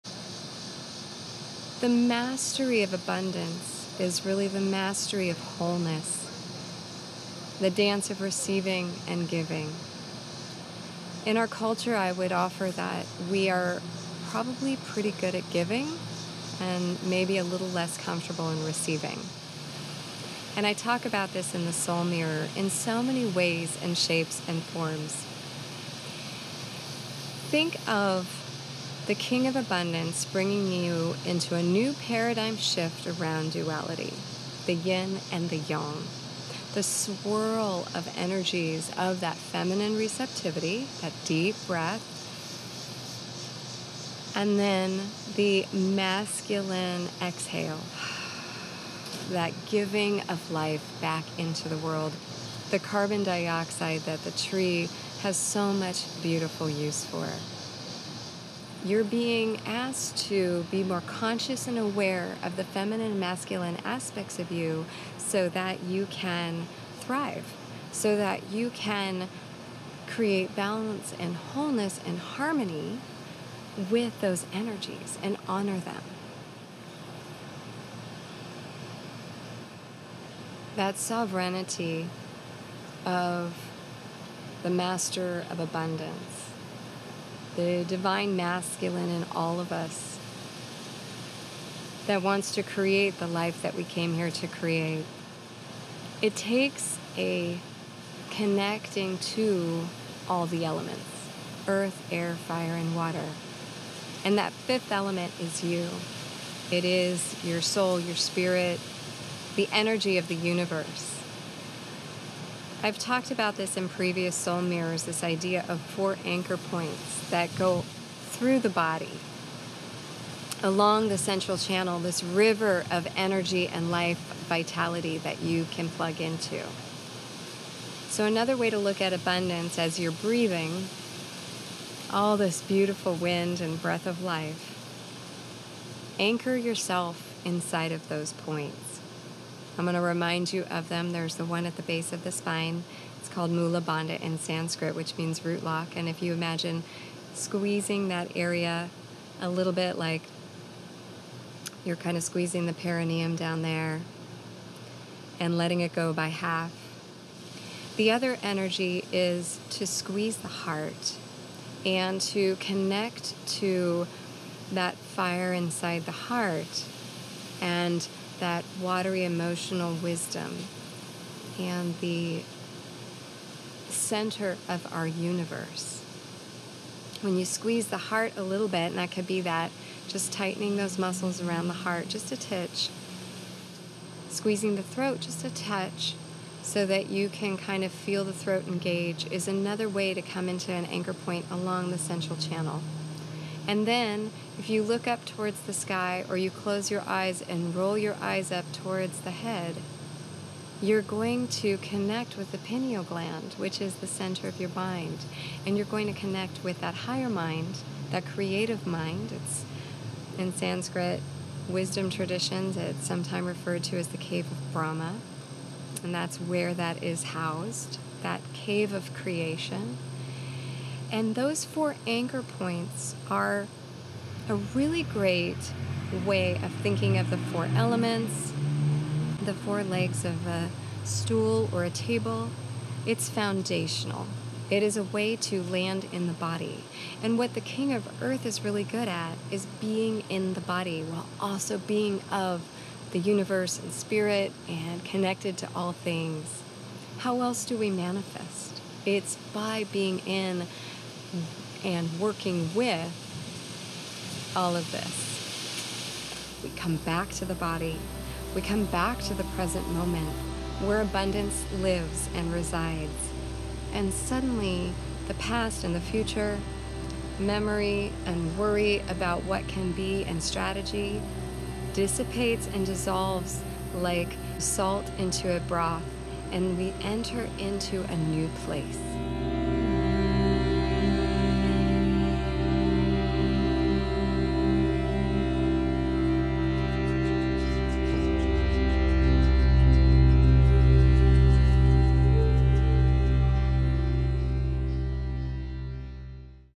Soul Mirror 38 Preview - Abundance
"Come back to the body, come back to the present moment, where abundance lives. Suddenly, the past and the future, memory and worry about what can be, dissipates and dissolves like salt in a broth and we enter into a new place." - Astara

Preview Astara in the 38th Soul Mirror to discover the global energies in this powerful new moon week. The new moon in Leo is Tuesday August 18th at 9:41pm Central Daylight Time (CDT) and is a grand reset helping you to come home to your own worth and enough-ness.

Learn from the King of Rainbows (Earth or Disks) - the master Manifester and Harvester - about the truth of abundance which arises from wholeness and soul esteem. Dive into the duality of creation to reconnect to the universal flow, receiving and generating prosperity as easy as the inhale and exhale. Practice a breathing technique to activate the third eye to bring you to coherence within your body and field. From there, you can healthfully manifest beauty on all levels.